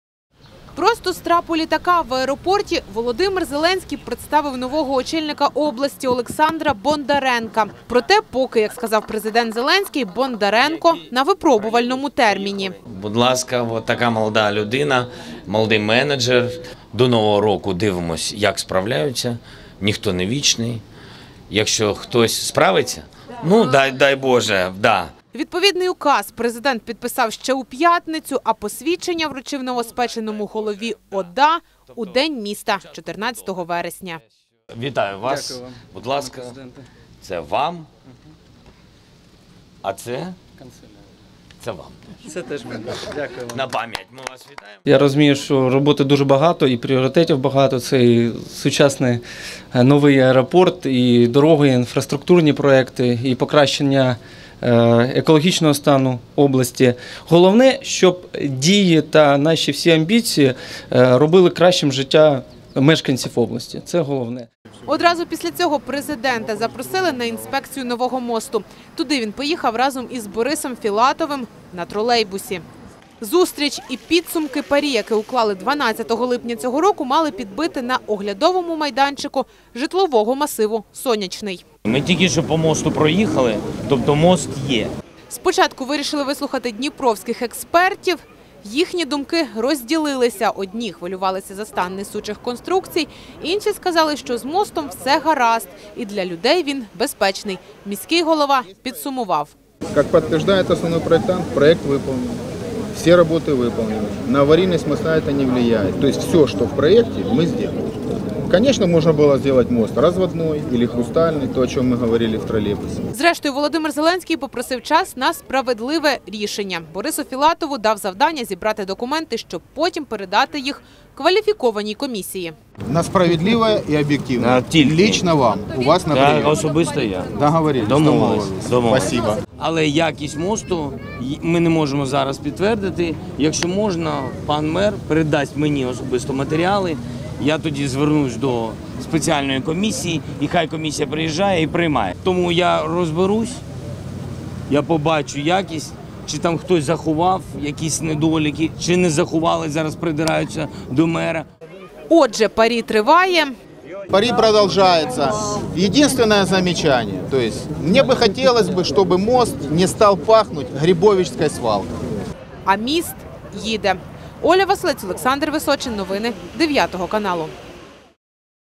Візит президента Зеленського до Дніпра: знайомство із новим головою ОДА та інспекція нового мосту
Просто з трапу літака, в аеропорті Володимир Зеленський представив нового очільника області Олександра Бондаренка. Проте поки, як сказав президент Зеленський, Бондаренко на

випробувальному терміні.